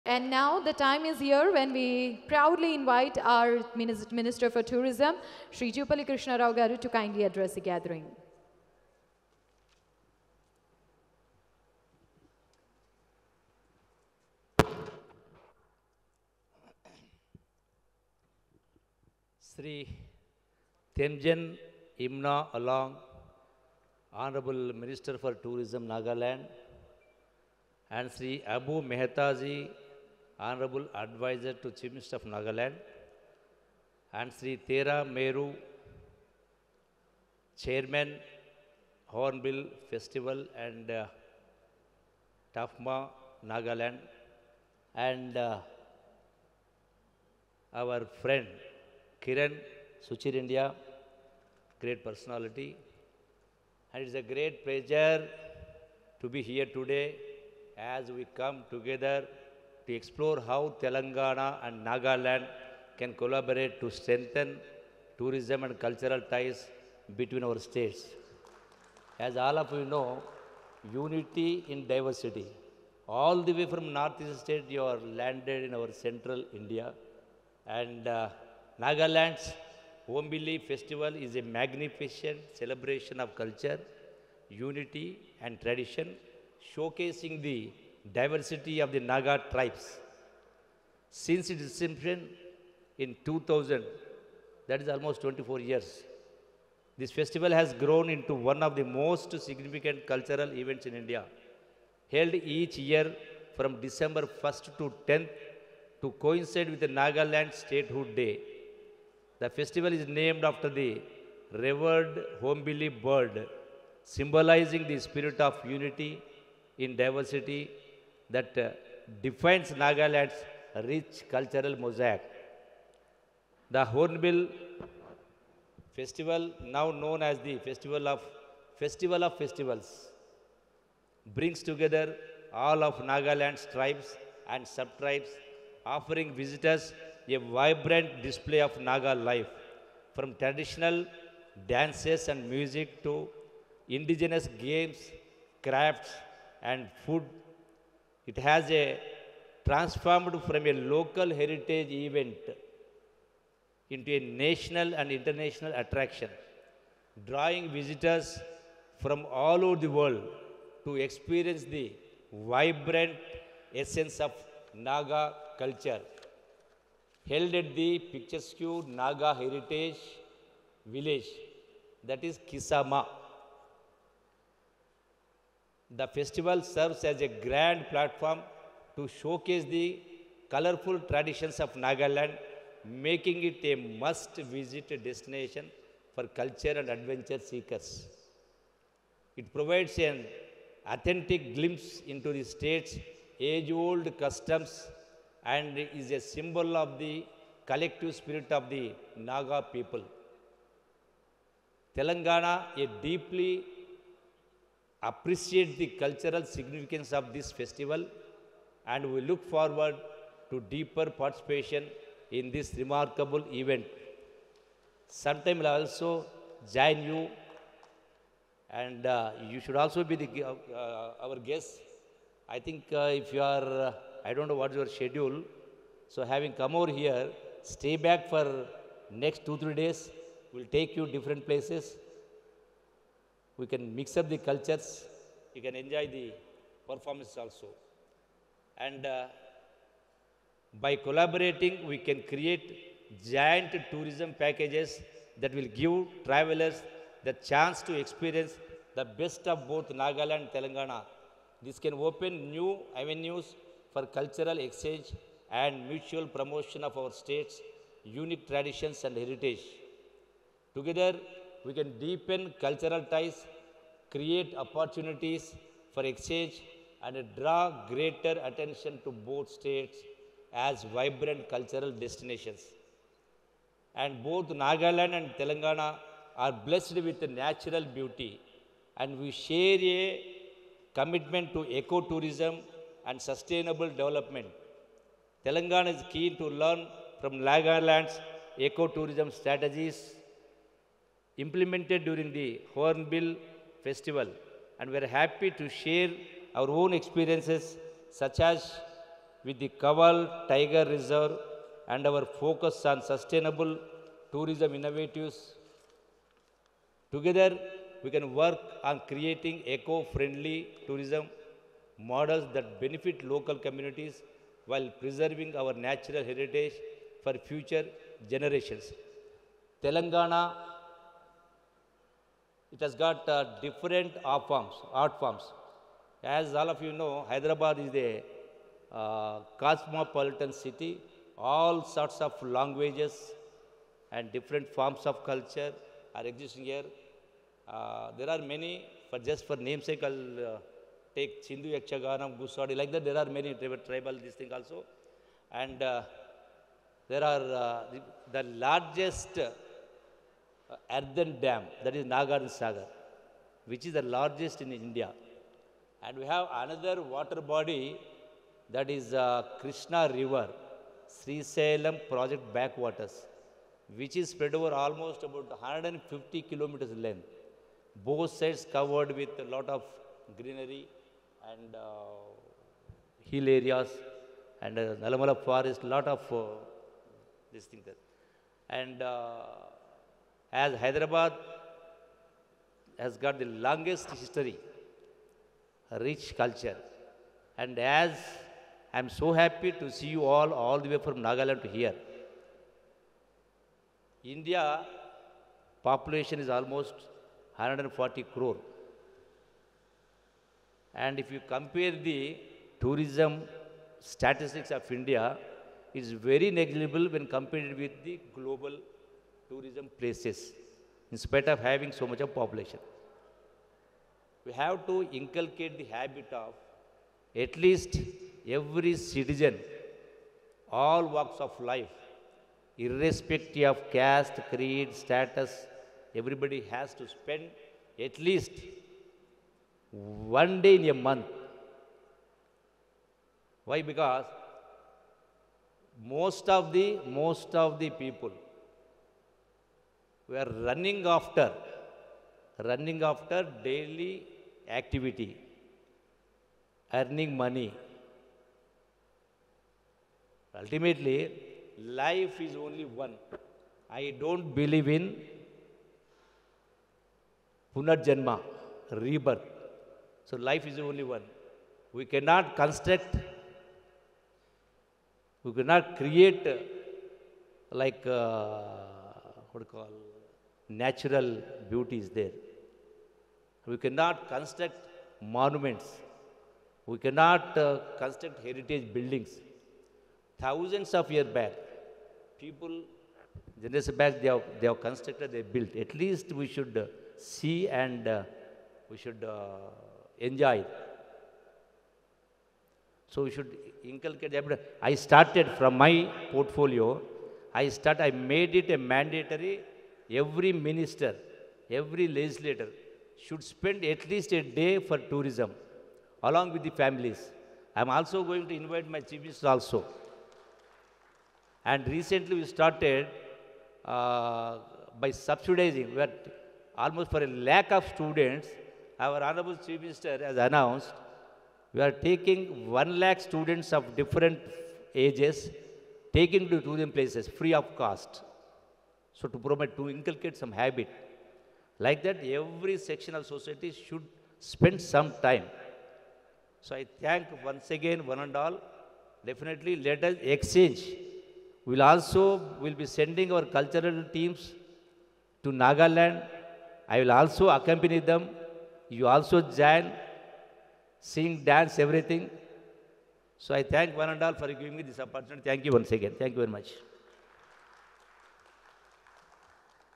Minister of Tourism Sri Jupally Krishna Rao Speech at National Press Conference | Shreyas Media
Watch Minister of Tourism Sri Jupally Krishna Rao Speech at National Press on ShreyasMedia